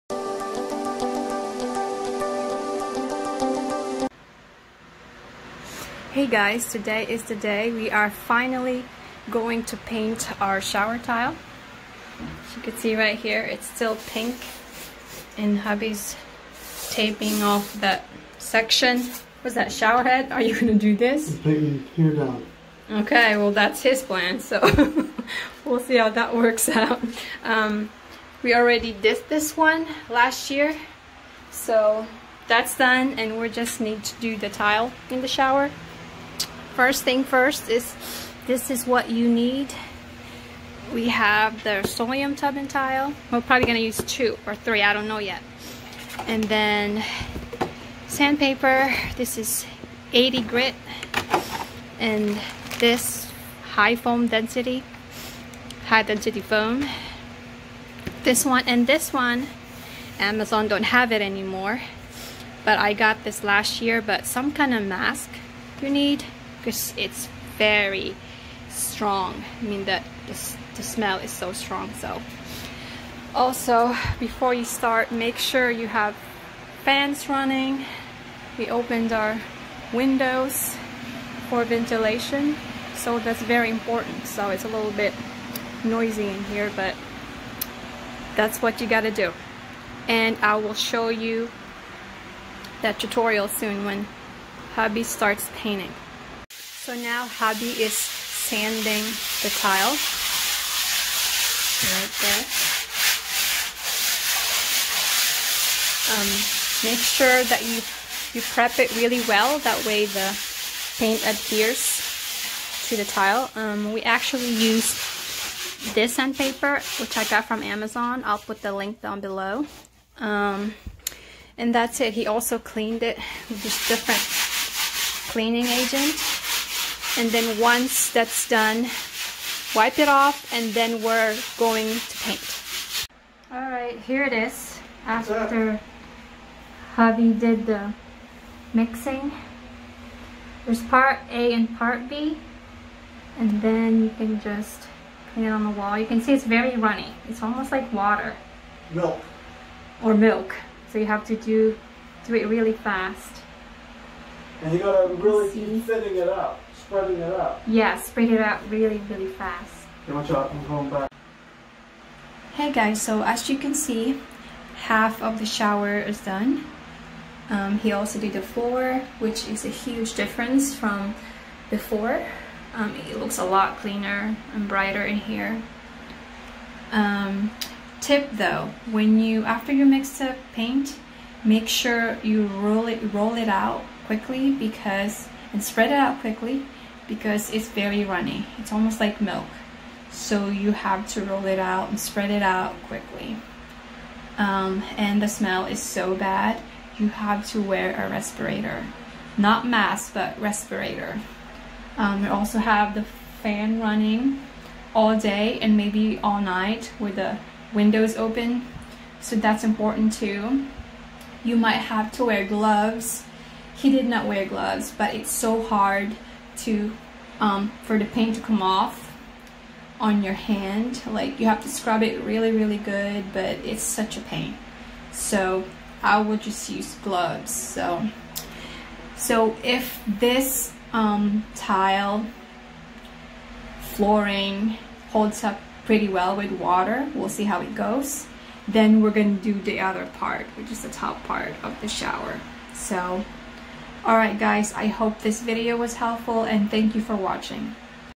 How To Paint Bathroom Tile
Do you have ugly bathroom tile? In this video I will show you how to update your dated tile using Rustoleum tub and tile paint.

Products used:

Rustoleum Tub and Tile Paint:

Foam Roller:

3M 60 grit Sandpaper:

(optional)

Peel and stick tile: